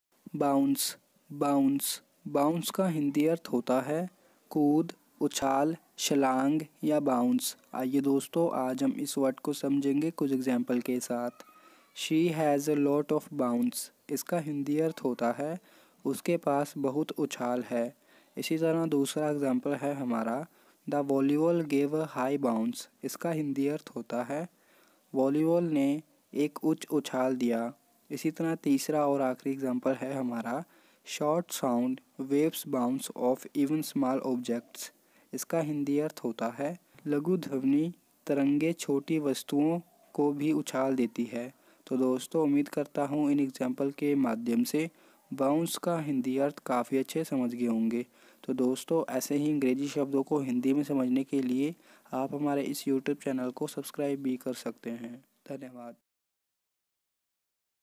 Bounce Meaning in Hindi/Bounce ka Matlab kya Hota hai
Bounce meaning in Hindi
Bounce ka Matlab kya Hota hai
Bounce ka Hindi Meaning
Bounce explained in detail.

इस विडियो में मैं  ,Bounce शब्द का इंग्लिश में अर्थ या मतलब क्या होता है इसके के बारे पूरी विस्तृत रूप से बताया .